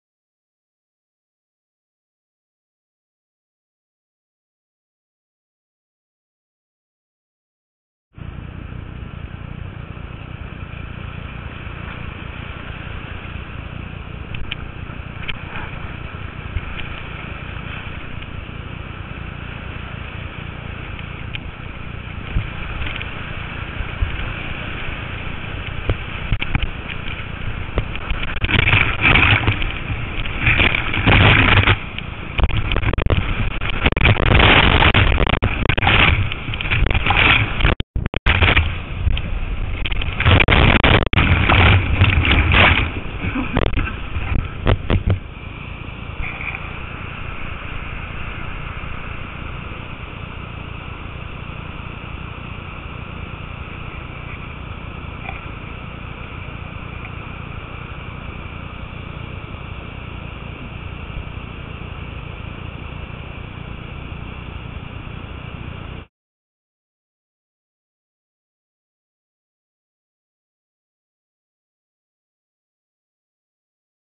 Lower Falls Riverboarding Carnage
POV cam of the first attempt at riverboarding Lower Falls on the Upper Ammonusac river in NH.  Also, surfing footage of some unintentional 360's and Barrel Rolls.  Too bad the person filming from shore botched the second attempt at running it as Kevin walked back up and stuck it (you didn't think he could let that first ugly descent count as a 1st D, did ya).